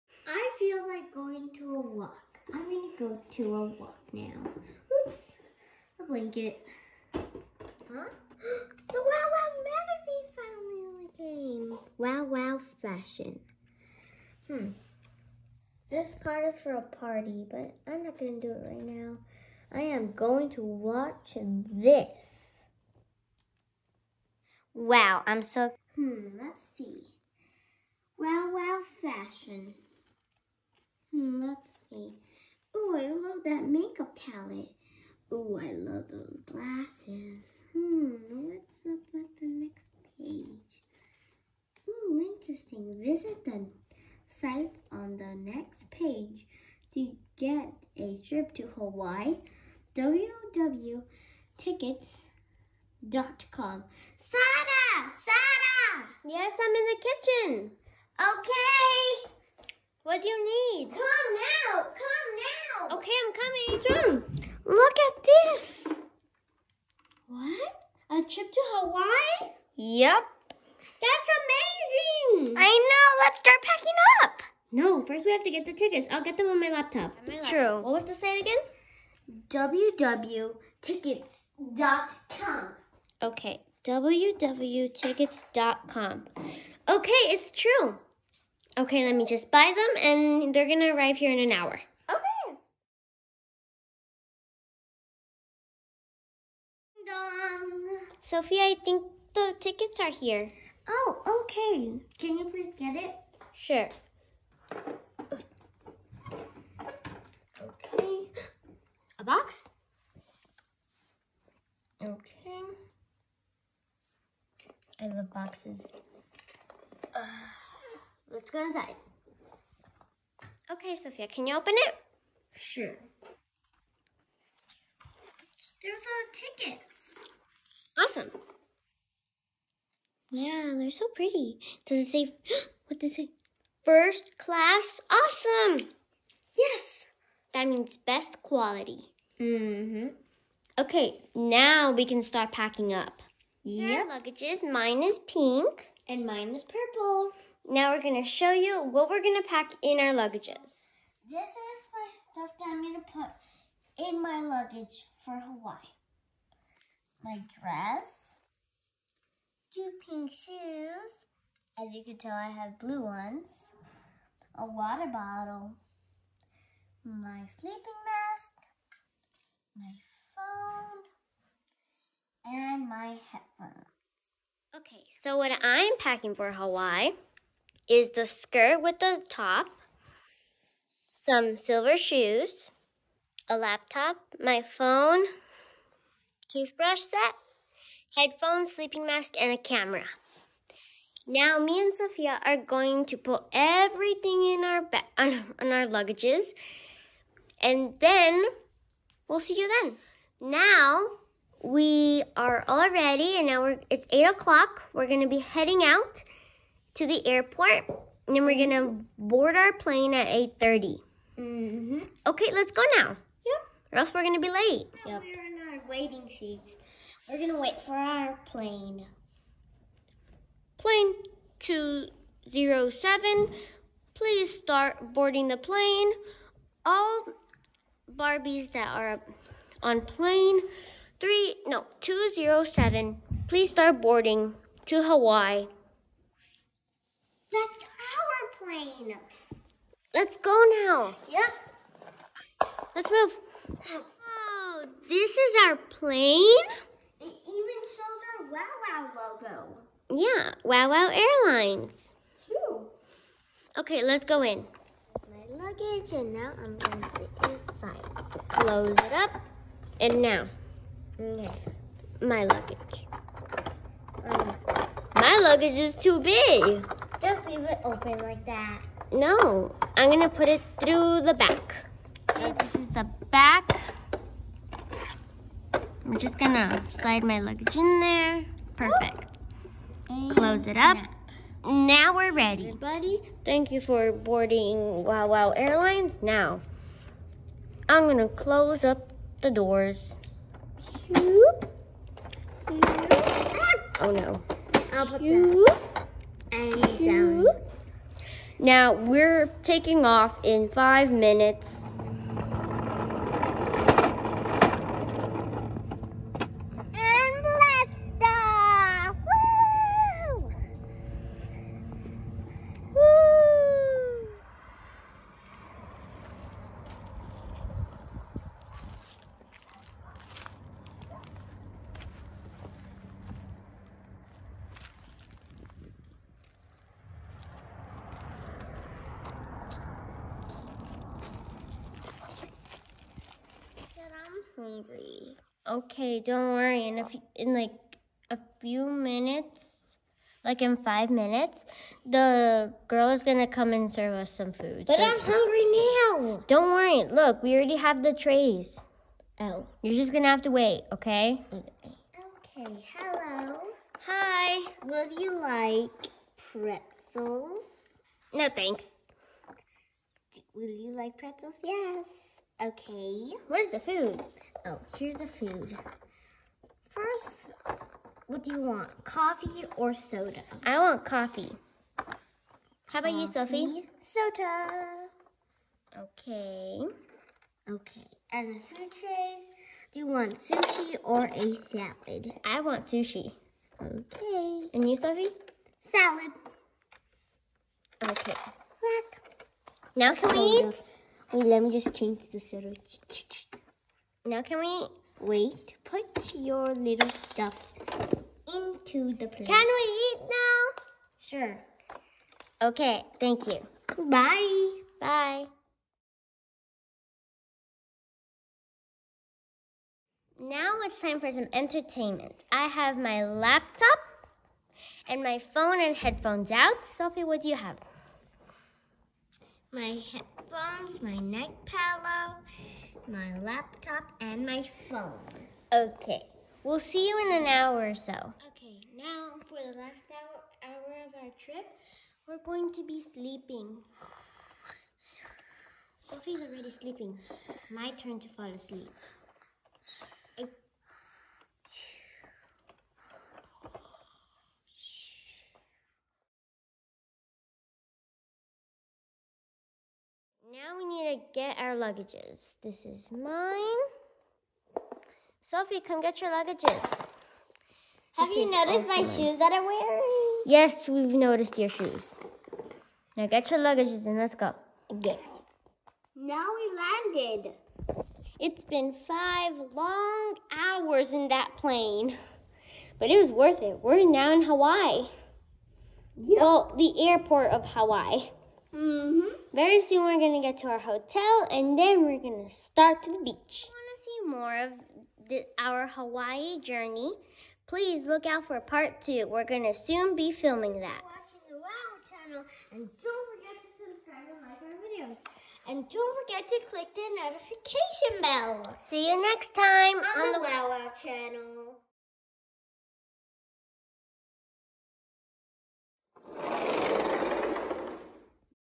Trip to Hawaii ( part 1 )
Zofia was reading the new fashion magazine and found out that her sister and herself only needed to buy tickets online and then they would be able to go on a on a 2 week vacation to Hawaii!!! then they packed up and went to the airport, then boarded awesome plane and zoomed off. Then they landed in the Hawaii airport.

( BE ON THE LOOKOUT FOR PART 2 TO SEE WHAT HAPPENS NEXT!!! )